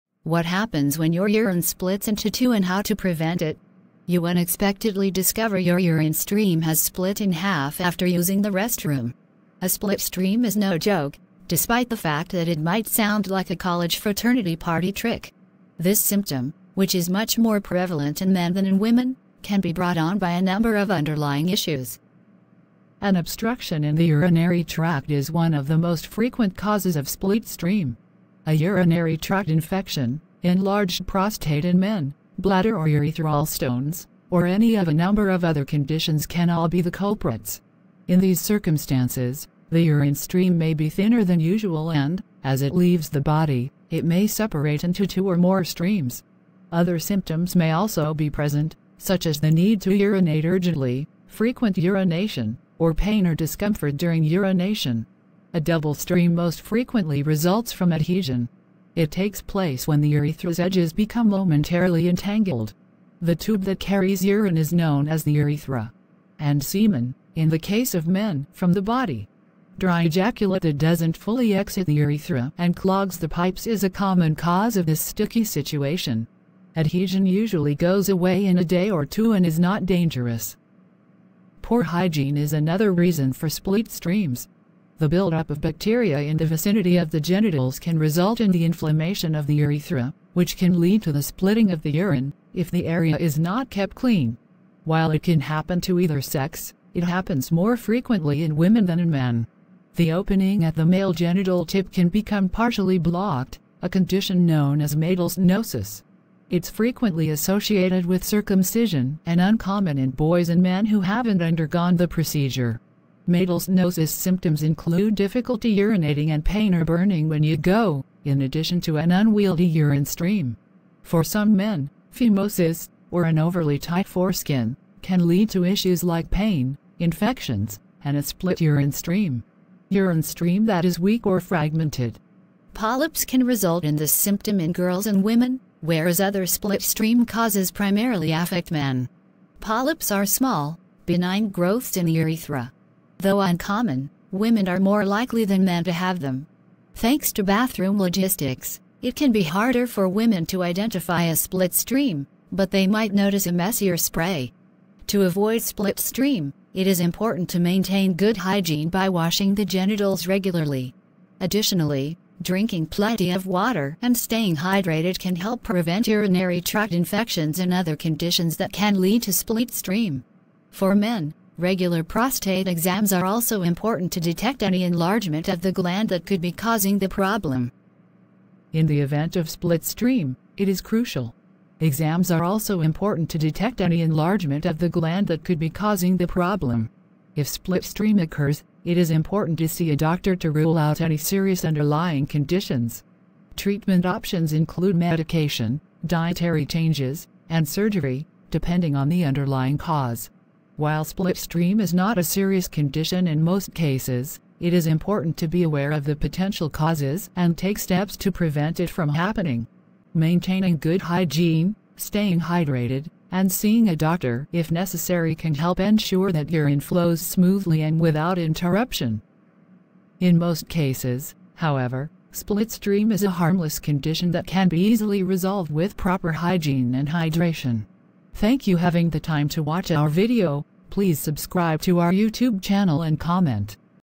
URINE SPLIT,WHAT HAPPENS
Relax and watch the video in full and learn.
What Happens When Your Urine Splits Into Two and How to Prevent It.

 You unexpectedly discover your urine stream has split in half after using the restroom. A split stream is no joke, despite the fact that it might sound like a college fraternity party trick.
 This symptom, which is much more prevalent in men than in women, can be brought on by a number of underlying issues.

 An obstruction in the urinary tract is one of the most frequent causes of split stream. A urinary tract infection, enlarged prostate in men, bladder or urethral stones, or any of a number of other conditions can all be the culprits. In these circumstances, the urine stream may be thinner than usual and, as it leaves the body, it may separate into two or more streams. Other symptoms may also be present, such as the need to urinate urgently, frequent urination, or pain or discomfort during urination.

 A double stream most frequently results from adhesion. It takes place when the urethra's edges become momentarily entangled. The tube that carries urine is known as the urethra.
 (and semen, in the case of men) from the body. Dry ejaculate that doesn't fully exit the urethra and clogs the pipes is a common cause of this sticky situation. Adhesion usually goes away in a day or two and is not dangerous.
 Poor hygiene is another reason for split streams. The buildup of bacteria in the vicinity of the genitals can result in the inflammation of the urethra, which can lead to the splitting of the urine, if the area is not kept clean. While it can happen to either sexe, it happens more frequently in women than in men.
 The opening at the male genital tip can become partially blocked, a condition known as meatal stenosis, according to Cleveland Clinic. It's frequently associated with circumcision and uncommon in boys and men who haven't undergone the procedure. Meatal stenosis symptoms include difficulty urinating and pain or burning when you go, in addition to an unwieldy urine stream. For some men, phimosis, or an overly tight foreskin, can lead to issues like pain, infections, and a split urine stream.
 Urine stream that is weak or fragmented. Polyps can result in this symptom in girls and women, whereas other split stream causes primarily affect men.  Polyps are small, benign growths in the urethra. Though uncommon, women are more likely than men to have them.  (Thanks to bathroom logistics, it can be harder for women to identify a split stream, but they might notice a messier spray. ).
 To avoid split stream, it is important to maintain good hygiene by washing the genitals regularly.  Additionally, drinking plenty of water and staying hydrated can help prevent urinary tract infections and other conditions that can lead to split stream.  For men, regular prostate exams are also important to detect any enlargement of the gland that could be causing the problem.
 In the event of split stream, it is crucial.
 Exams are also important to detect any enlargement of the gland that could be causing the problem.
 If split stream occurs, it is important to see a doctor to rule out any serious underlying conditions. Treatment options include medication, dietary changes, and surgery, depending on the underlying cause.
 While split stream is not a serious condition in most cases, it is important to be aware of the potential causes and take steps to prevent it from happening.  Maintaining good hygiene, staying hydrated, and seeing a doctor if necessary can help ensure that urine flows smoothly and without interruption.
.  In most cases, however, split stream is a harmless condition that can be easily resolved with proper hygiene and hydration.